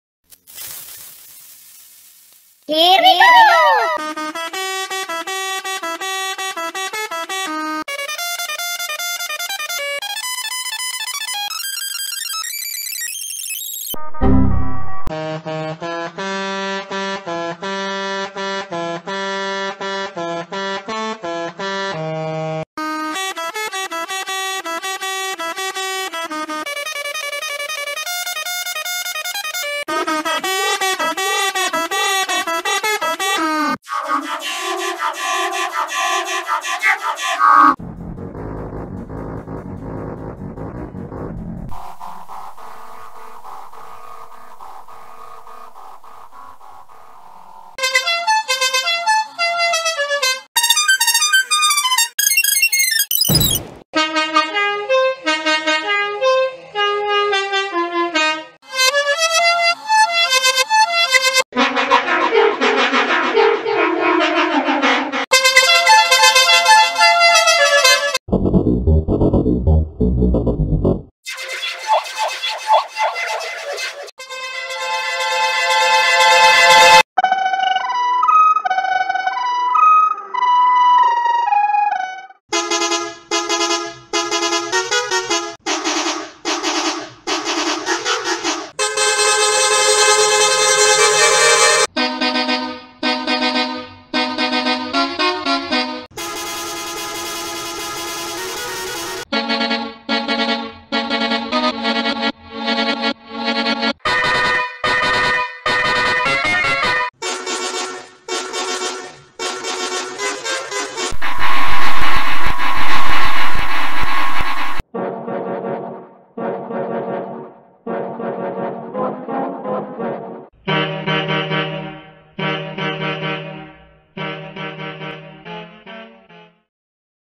50 "Bus Horn” Sound Variations in 125 Seconds
❗Top buses are here for you to serve, m'lord!
What effect do you like most?

Pleace help me find out what effect is the funniest - comment under this video⤵

*All Variations in this Video are created by me TOP BANANA.